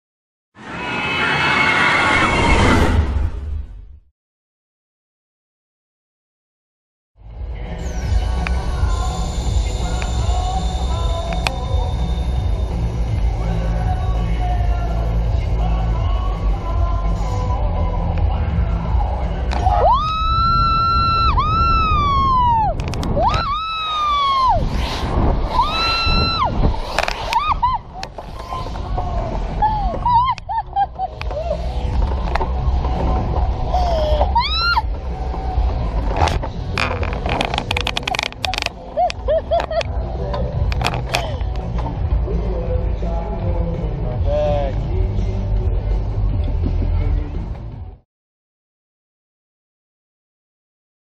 Super Sling on-ride 4K POV Bigfoot Fun Park
Standing 200 feet (61 m) tall, Bigfoot Action Tower opened in May 2017 and offers two adrenaline-pumping rides. Designed by the legendary Stan Checketts, the founder of S&S, and manufactured by his Soaring Eagle company, one side of the structure is this unique slingshot attraction. Unlike other skyshot rides, this one does not use spring or bungee mechanisms but a pneumatic cable launch. What sets this apart from the others is the open saddle style back-to-back seating as you soar into the sky and tumble head-over-heels back to the ground.
Check out the off-ride video we posted to see more of this thrilling slingshot ride and how it works
Bigfoot Fun Park is located in Branson, Missouri.